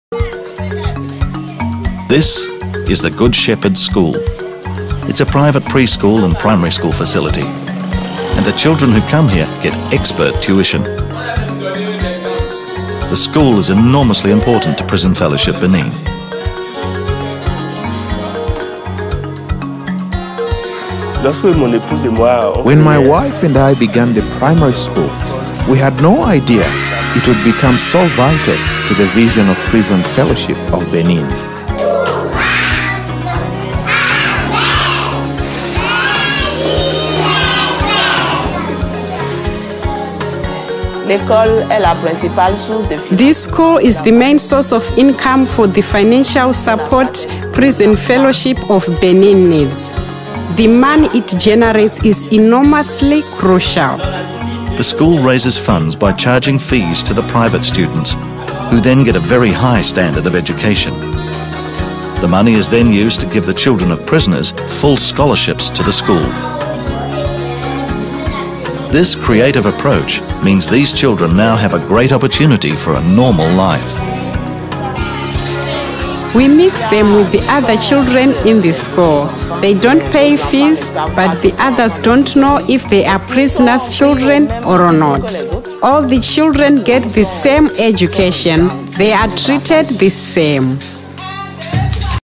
Prison Fellowship Benin-Family Support and Education (French Subtitles)
Prisonniers families' ; sont octroyés l'aide pour compenser un membre de la famille incarcéré